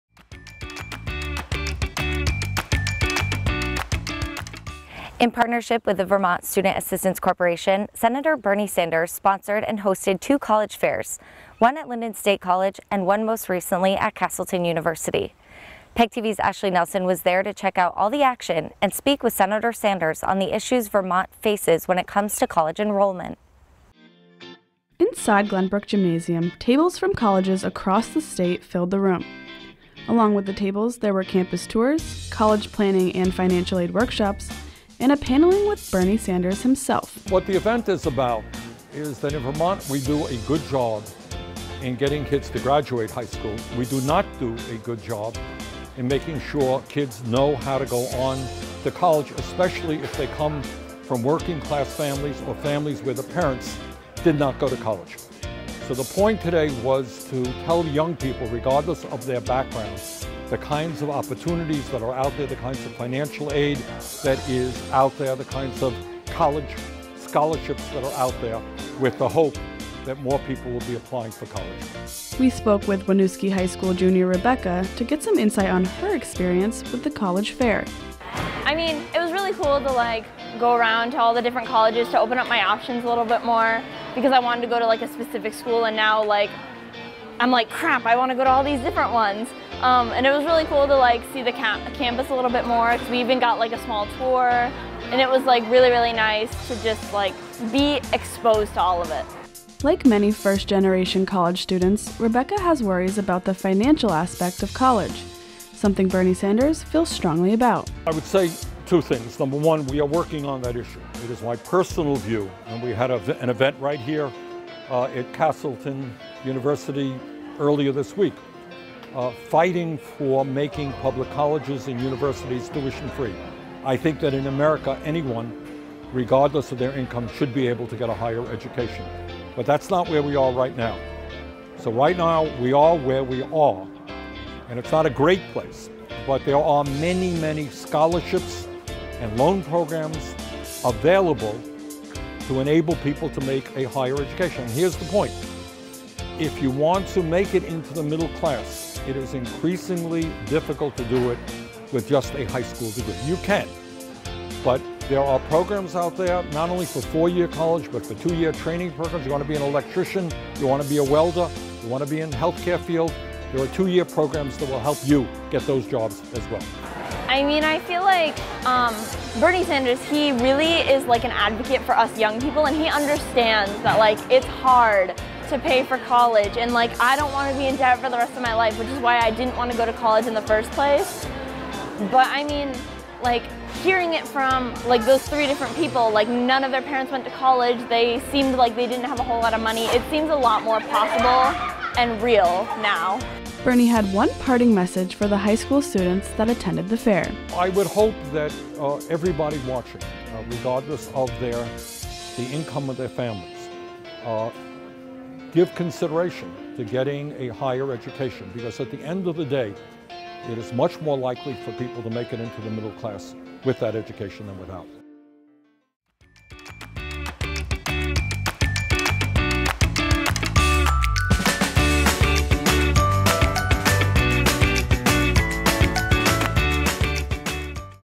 College Fair with Bernie Sanders
We visited Castleton University to take a look inside the college fair with Bernie Sanders